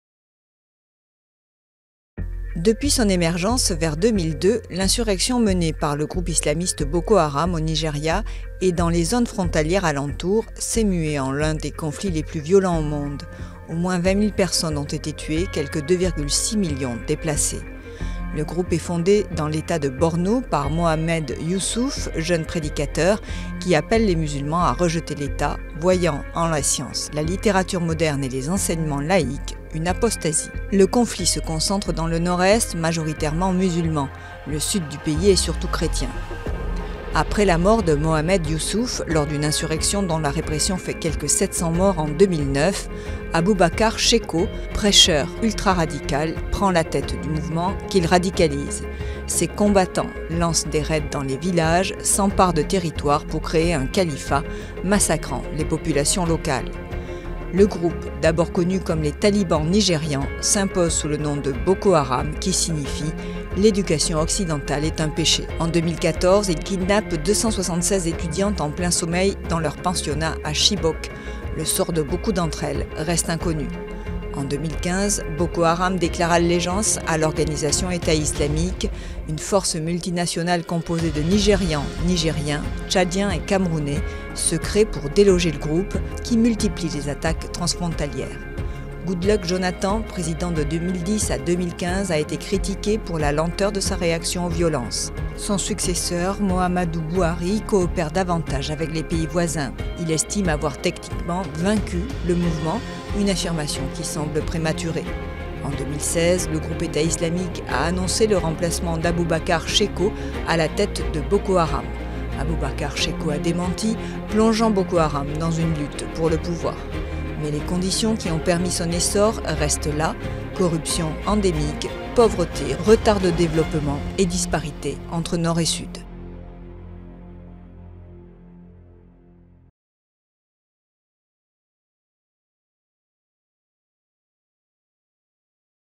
Boko Haram
Depuis son émergence vers 2002, l’insurrection menée par le groupe islamiste Boko Haram, au Nigeria et dans les zones frontalières alentour, s’est mué en l’un des conflits les plus violents au monde. Au moins 20.000 personnes ont été tuées, quelque 2,6 millions déplacées.VIDEOGRAPHIE